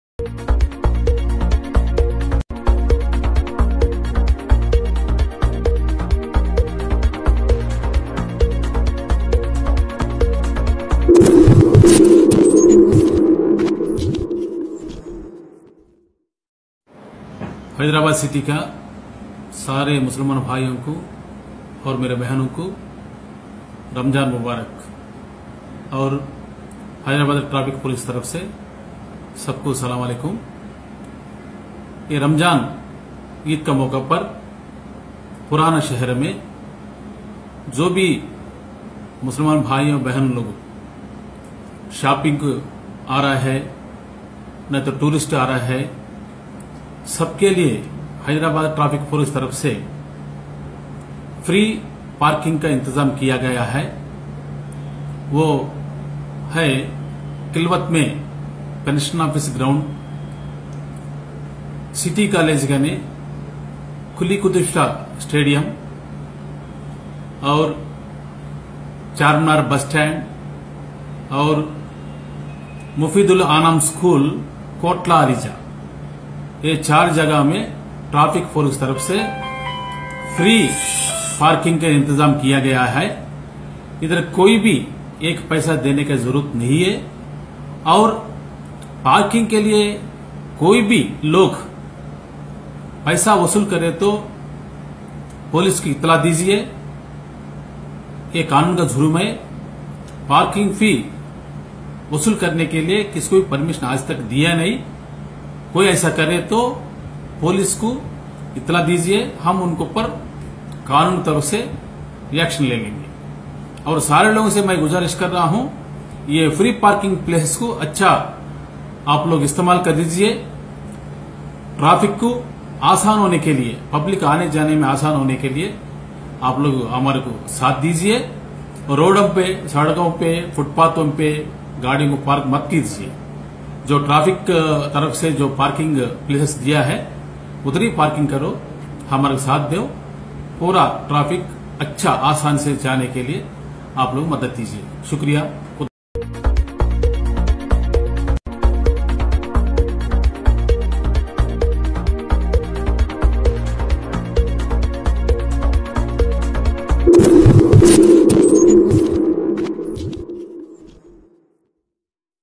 DCP Traffic Southzone Babu Rao Warning to Parking Mafia in Old city.
DCP Traffic Southzone Babu Rao Warning to Parking Mafia in Olcity.On the view of the Holy month of Ramazan 2019, the traffic police South Zone Hyderabad city have arranged free parking in the below mention places to ease the visitors and other general public who are coming to old city for prayers as well as to do shopping. The free parking places are as follows
1. Old Pension Office Khilwat.
2. Khilwat Play ground.
3. Charminar Bus terminal.
4. Quli Qutub Shah Stadium near City Collage.
5. Mufeed Ul Anam School near Etabaar Chowk. (Mir Chowk)
The above mentioned parking places are running free of cost.